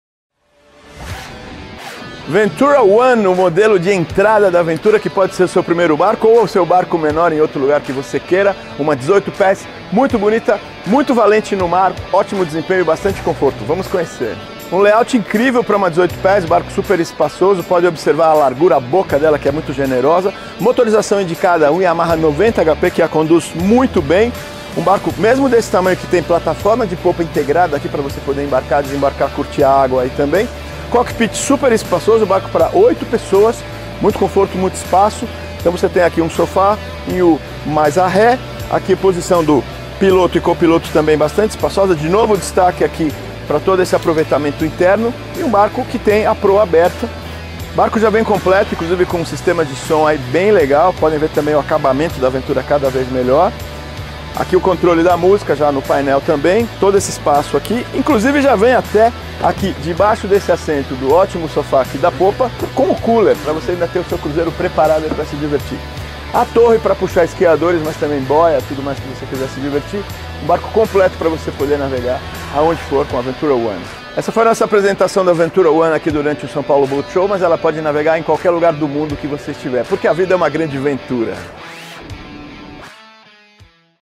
V one a número um, por sua leveza e performance.
Na V ONE você encontra leveza, performance e design acompanhando as novas tendências internacionais para a sua embarcação, com capacidade para 8 pessoas, com excelente espaço interno e conforto.